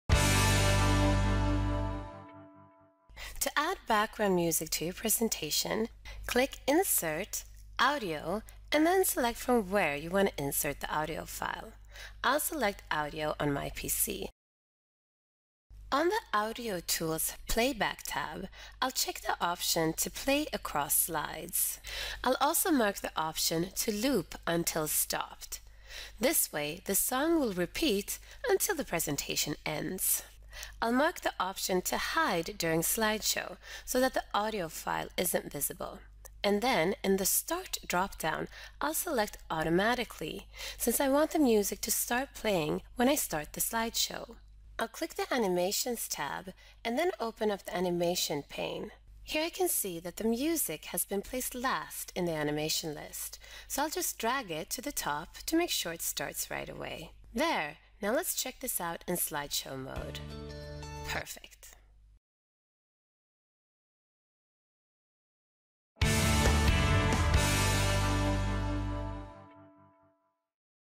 How to add music to your presentation in PowerPoint 2013?
In this short video, you will learn how to insert an audio file to your slide to play music throughout the presentation.

Produced by Business Productivity.
Instructor: Ulrika Hedlund.

Check out our How-to sites of Free Office 2013 video tutorials